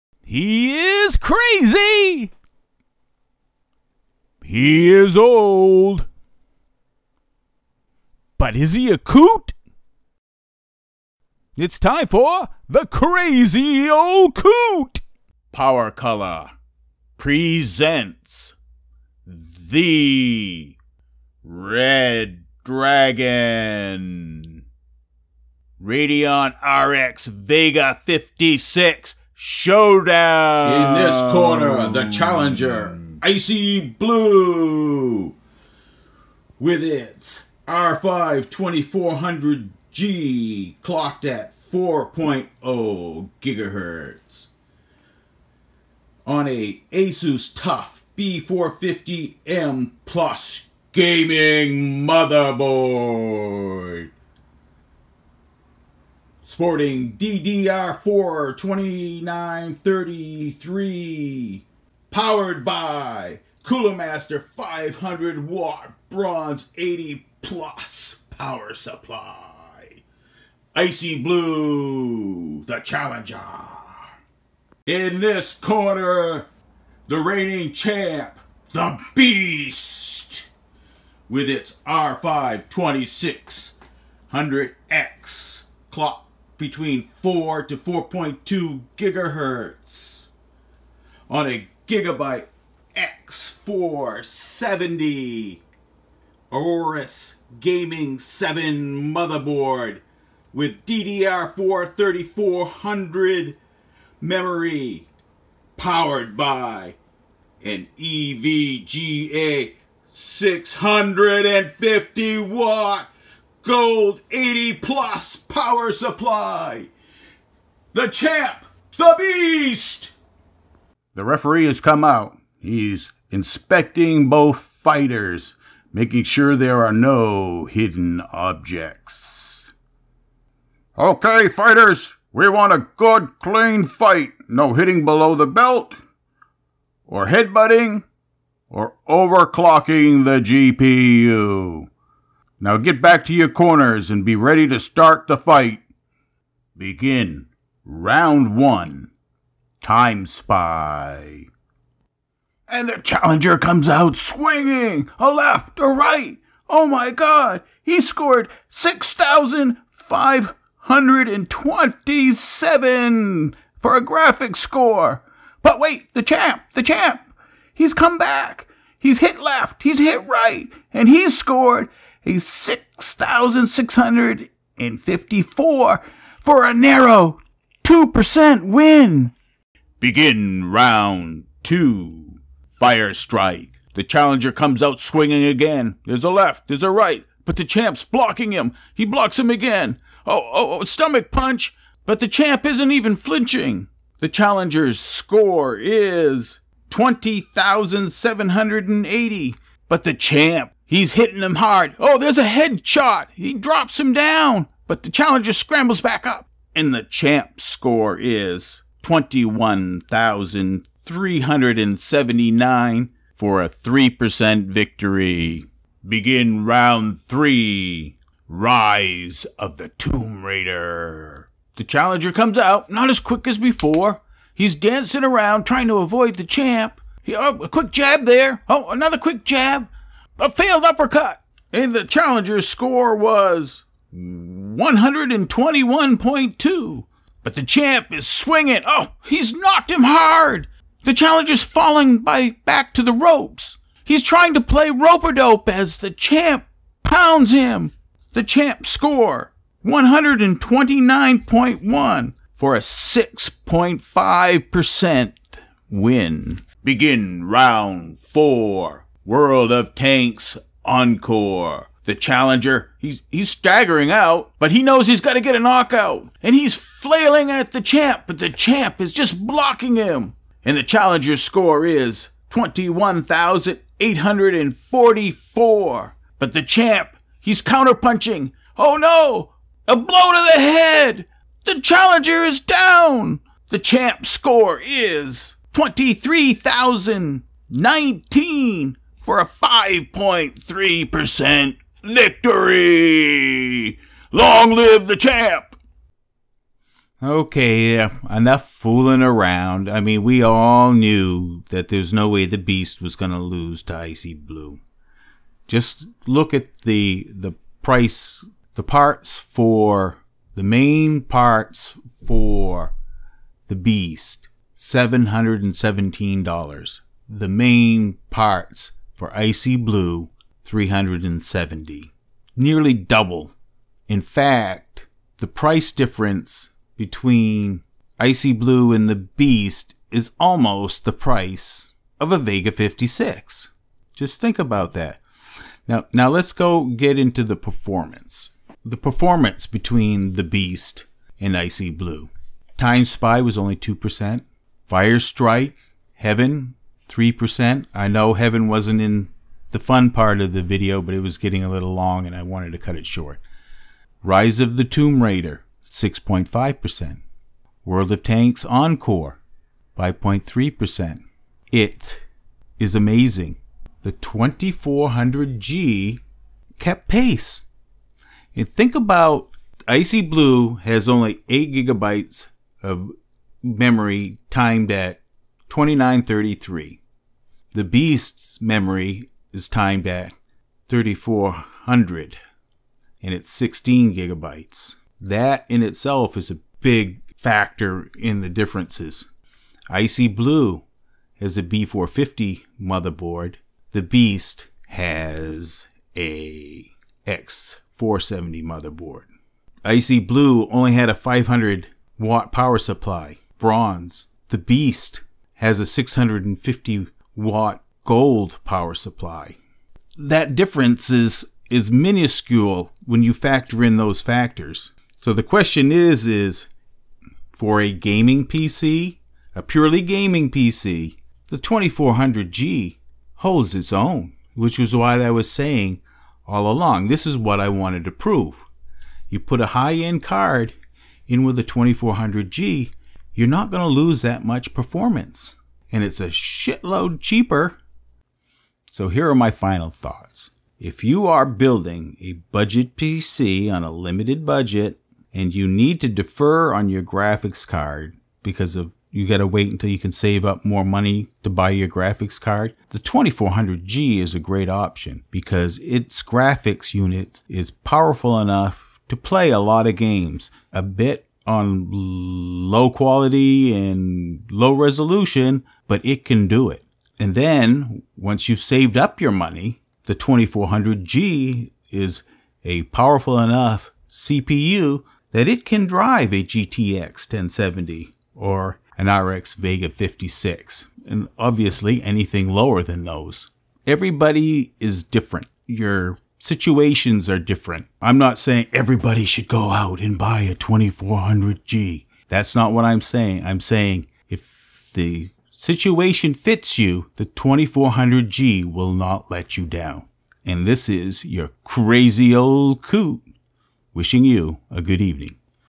PowerColor Red Dragon Vega 56 Showdown - R5 2600X vs R5 2400G
Just how close can the R5 2400G come to the performance of a R5 2600X using a PowerColor Red Dragon Vega 56 in 5 tests?

for those who want  to skip the fun and go to the analysis 4:58

Icy Blue
R5 2400G Overclocked to 4GHz
Asus Tuf B450M Plus Gaming
8GB 2933 DDR4
Cooler Master 500Watt Bronze

The Beast
R5 2600X no overclock
Gigabyte X470 Aorus Gaming 7
16GB DDR4 3400 DDR4
EVGA 650 Watt Gold